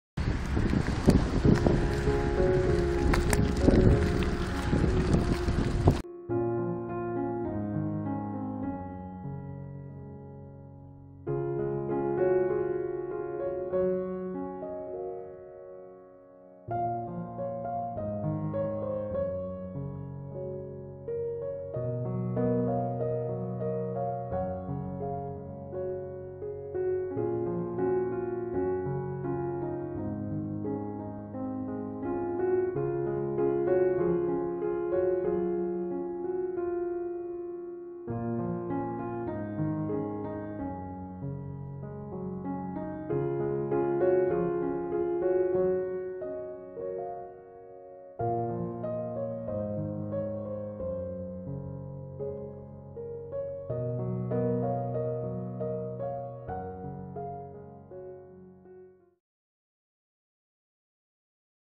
プロローグ 園路へ
雁来から豊平川サイクリング園路へのアクセス紹介です。